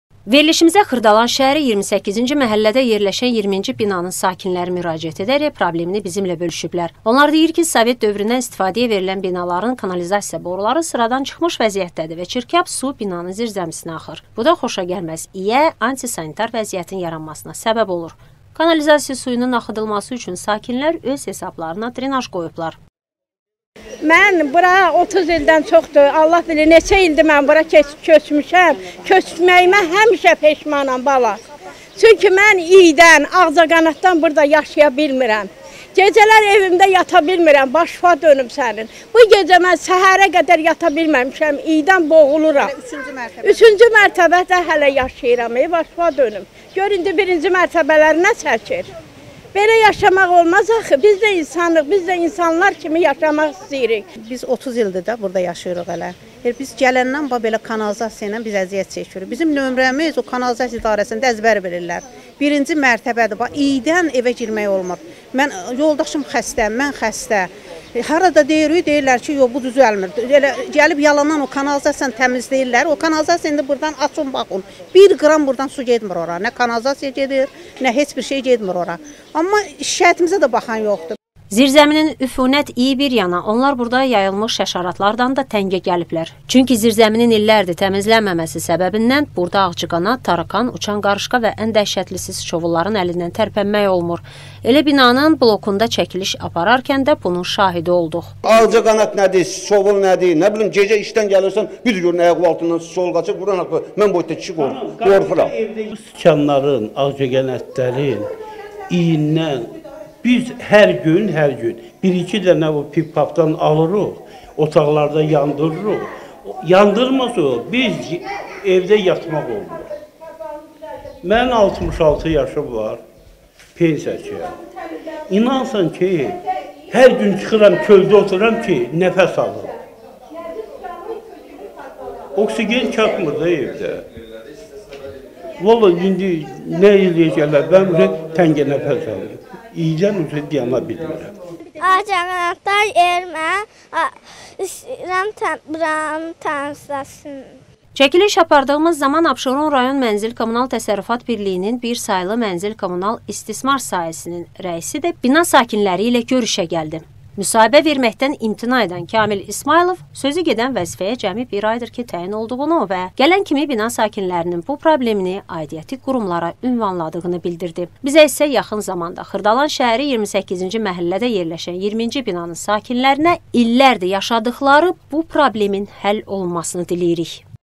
Zirzəmi problemi yaşayan xırdalanlılar
Bina sakinlərinin problemi aidiyyəti qurumlara ünvanlanıb